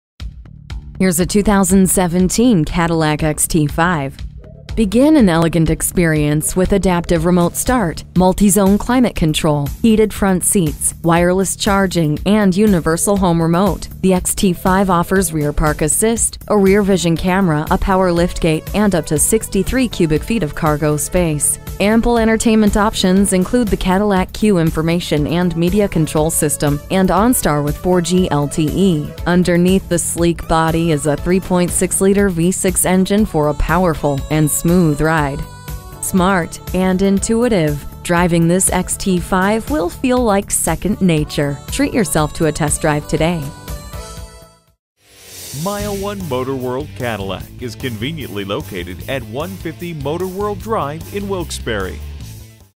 New 2017 Cadillac XT5 Wilkes Barre PA Scranton, PA C4361 - SOLD
Year: 2017
Make: Cadillac
Model: XT5
Trim: Luxury
Engine: 3.6L V6 DI VVT
Transmission: 8-Speed Automatic
Color: Gray
Interior: Sahara Beige
Stock #: C4361
VIN: 1GYKNDRS7HZ285089
All Wheel Drive! Gasoline! This 2017 XT5 is for Cadillac enthusiasts who are yearning for that rare catch.

Address:
150 Motor World Dr